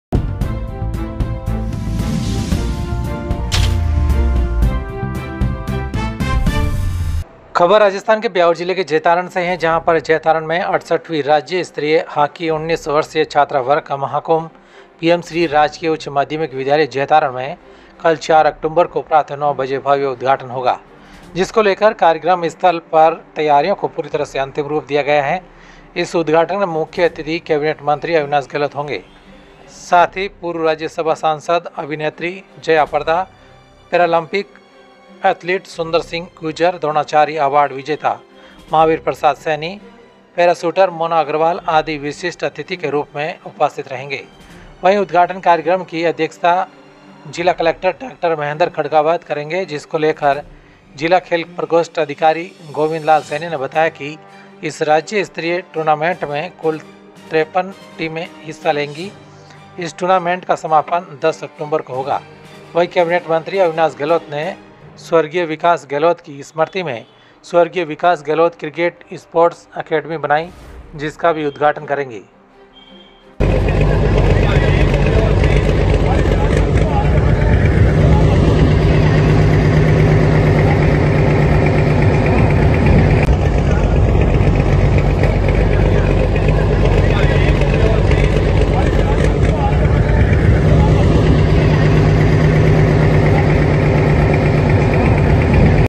ब्यावर_जैतारण_68वी राज्य स्तरीय हॉकी छात्रा वर्ग का महाकुंभ का उद्घाटन कल_कुल 53 टीमें लेगी भाग ।
68वी राज्य स्तरीय हॉकी छात्रा वर्ग का महाकुंभ का उद्घाटन कल,कुल 53 टीमें लेगी भाग

जैतारण में 68वीं राज्य स्तरीय हॉकी 19 वर्षीय छात्रा वर्ग का महाकुंभ पीएम श्री राजकीय उच्च माध्यमिक विद्यालय जैतारण में कल 4 अक्टूबर को प्रातः 9 बजे भव्य उद्घाटन होगा। इस उद्घाटन में मुख्य अतिथि कैबिनेट मंत्री अविनाश गहलोत होगे । साथ ही पूर्व राज्य सभा सांसद अभिनेत्री जयप्रदा,पैरालंपिक एथलीट सुन्दर सिंह गुर्जर,द्रोणाचार्य अवार्ड विजेता महावीर प्रसाद सैनी, पैरा शूटर मोना अग्रवाल आदि विशिष्ट अतिथि के रूप में उपस्थित रहेेगे। वही उद्घाटन कार्यक्रम की अध्यक्षता जिला कलक्टर डॉ महेन्द्र खड़गावत करेगें। जिसको लेकर जिला खेल प्रकोष्ट अधिकारी गोविन्द लाल सैनी ने बताया इस राज्य स्तरीय टुर्नामेन्ट में कुल 53 टीमें हिस्सा लेगी। इस टुर्नामेन्ट का समापन 10 अक्टुबर को होगा। वही कैबिनेट मंत्री अविनाश गहलोत ने स्वर्गीय विकास गहलोत की स्मृति में स्वर्गीय विकास गहलोत क्रिकेट स्पोर्टस् एकेडमी बनाई। जिसका भी उद्घाटन करेगें।